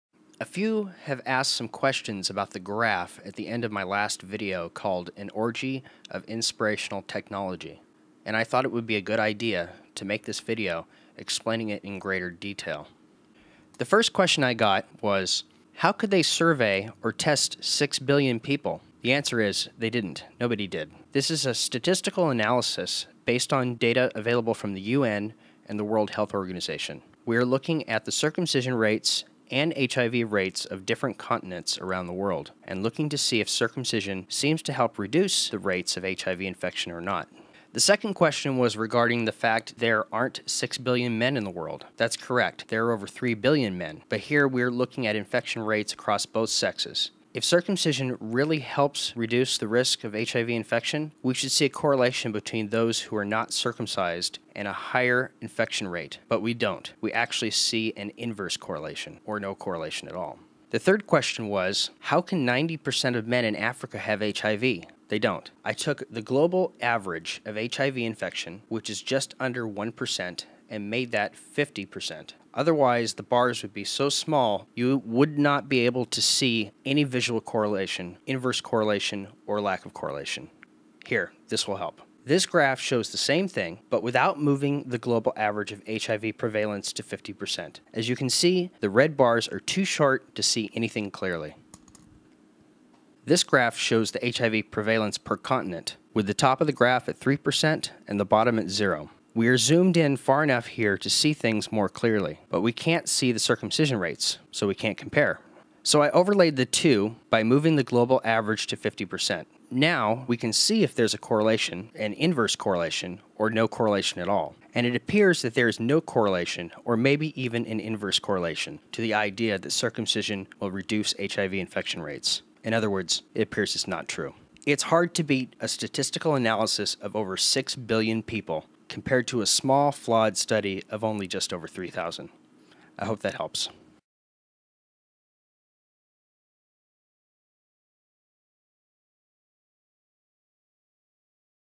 Explaining the HIV Graph
A few people had trouble understanding the graph, so I thought I'd make a video to help explain it.